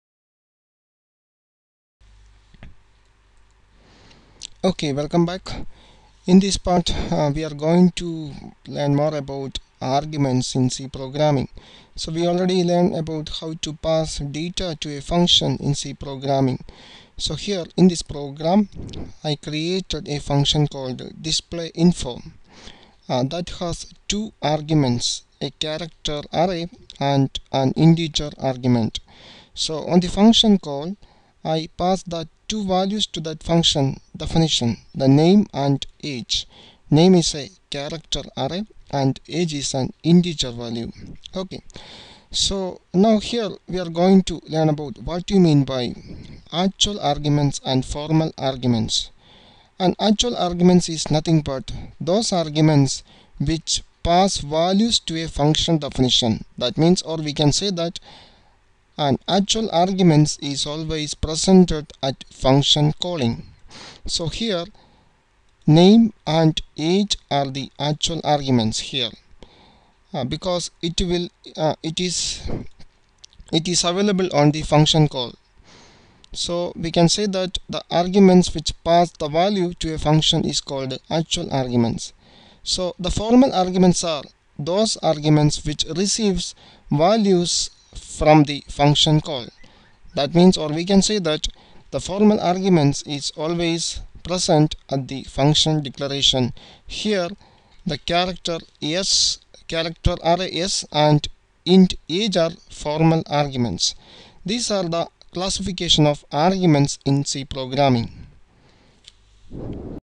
C Programming - 26 - Actual and Formal arguments
Classifications of arguments or parameters of a method in c.